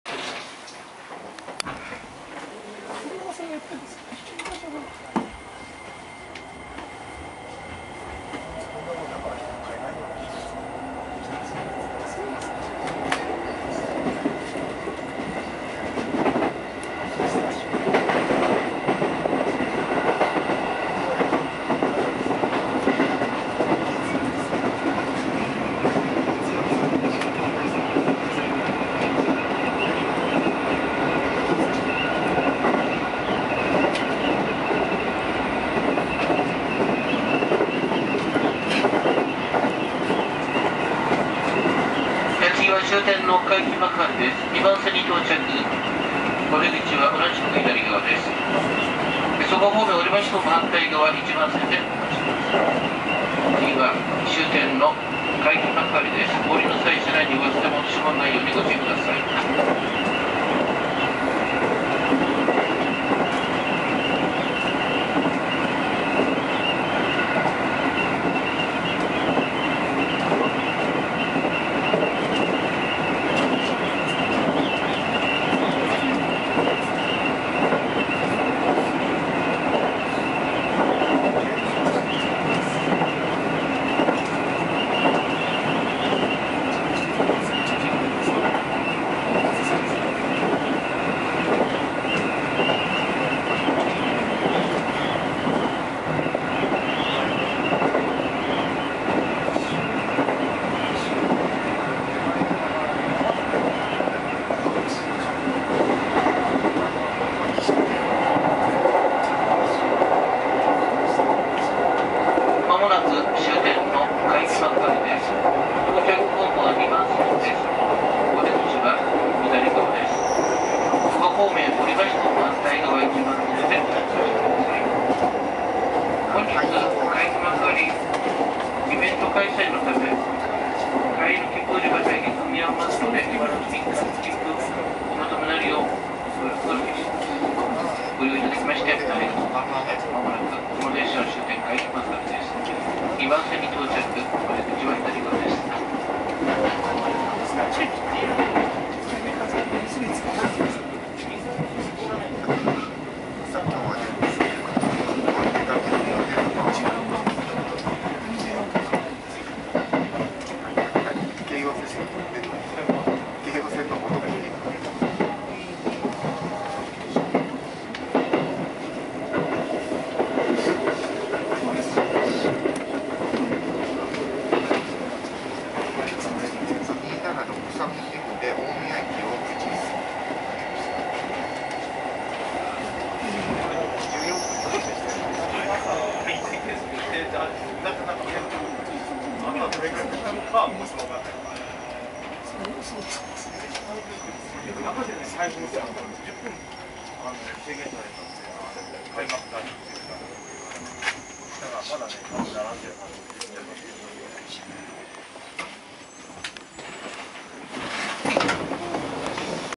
前面展望・新習志野⇒海浜幕張／しもうさ号205系M17編成＠SONY DSC-WX1
2011年2月6日（日）10時25分頃に撮影を開始した、しもうさ号「海浜幕張」行き先頭車（クハ205-10）の、新習志野から海浜幕張までの前面展望です。海浜幕張では、京葉線20周年記念ヘッドマークを付けたE233系5000番台ケヨ501編成の各駅停車「東京」行きと並びます。